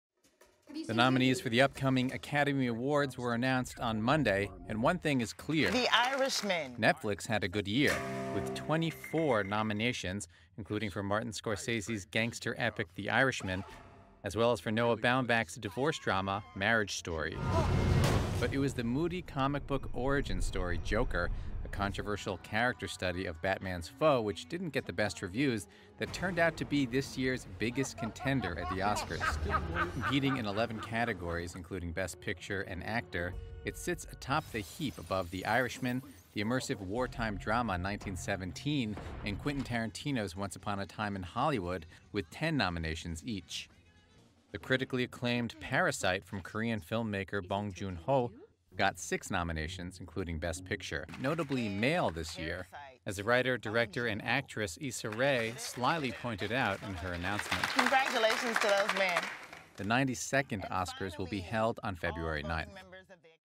「ジョーカー」最多11部門、「アイリッシュマン」は10部門ノミネート 米アカデミー賞
米映画芸術科学アカデミーは13日、第92回アカデミー賞の候補を発表した。最多ノミネートは、米漫画「バットマン」の人気悪役の誕生秘話を描いた「ジョーカー」（トッド・フィリップス監督）で、作品賞、主演男優賞など11部門。米配信大手ネットフリックスが製作した、米マフィア社会の人間模様がテーマの「アイリッシュマン」（マーティン・スコセッシ監督）など3作品が10部門で続いた。（ロイター）2020年1月14日公開